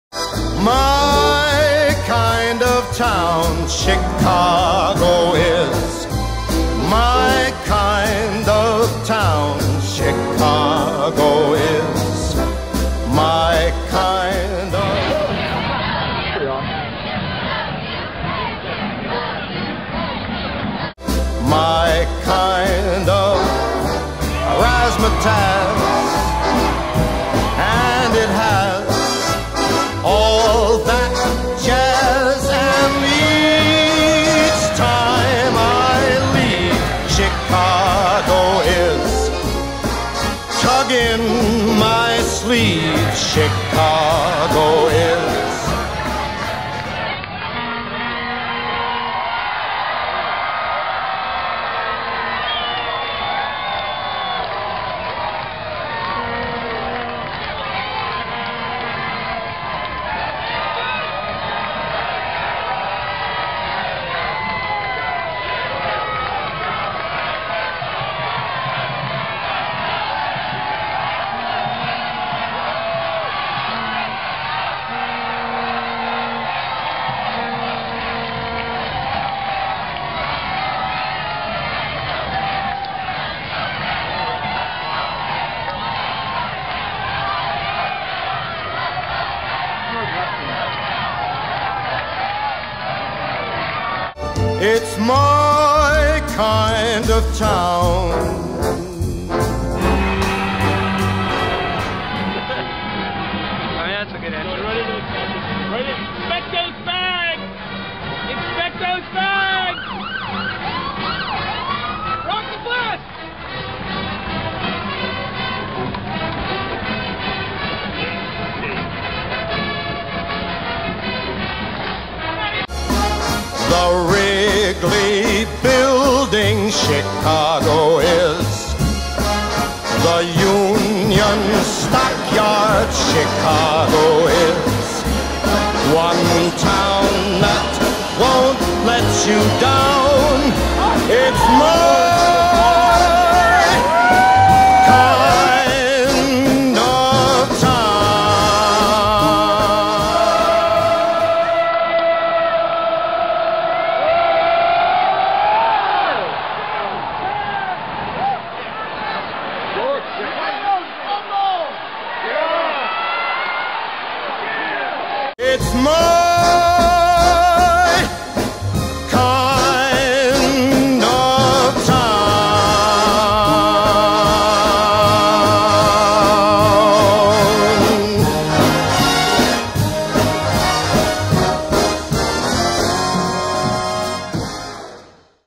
Northwestern v. Illinois at Wrigley Field
A look at the Northwestern v. Illinois game on November 20th at Wrigley Field, including the teams arriving at the stadium. Not pictured: The 516 yards of rushing Illinois put up on the WIldcats. Final: Illinois 48, NU 27.